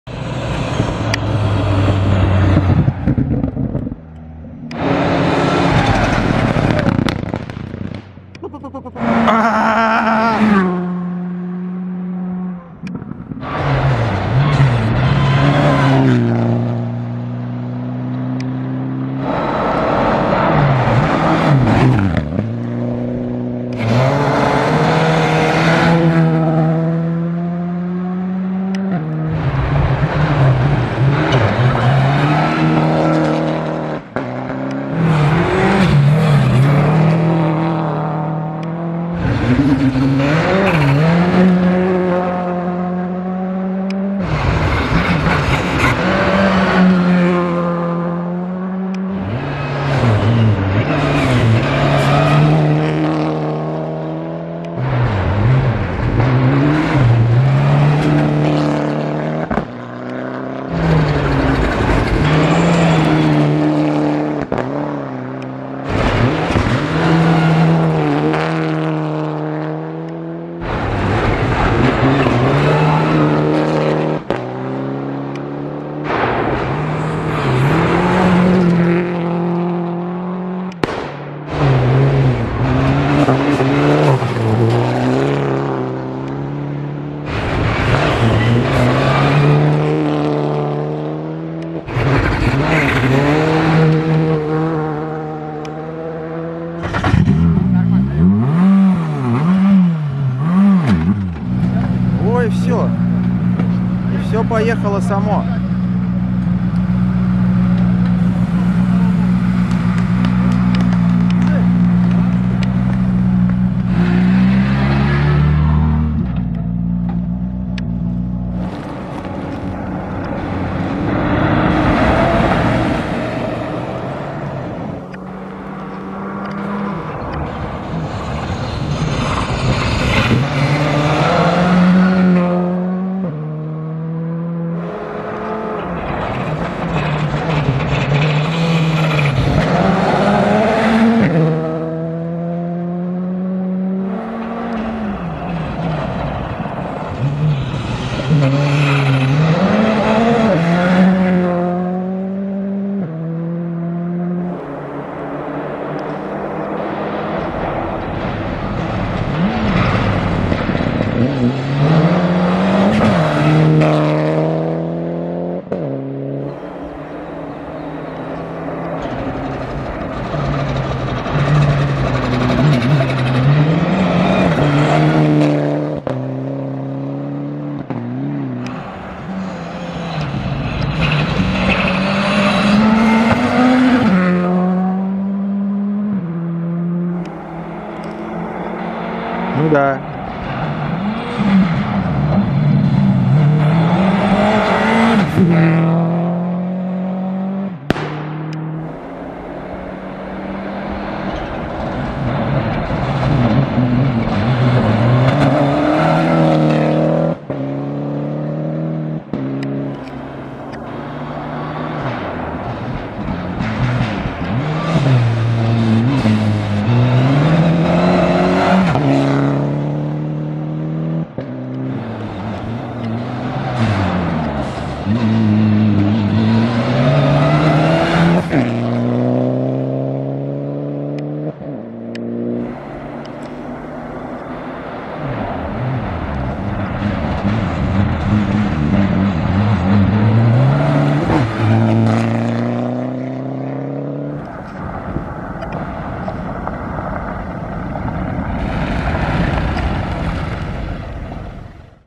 Квалификация. Ралли "Уралхим Псков" 2023. Видеосъемка Михаила Жукова.
III этап чемпионата России по ралли и II этап Кубка содружества «Трофей Александра Невского». Монтаж видео Алисы Колесниковой..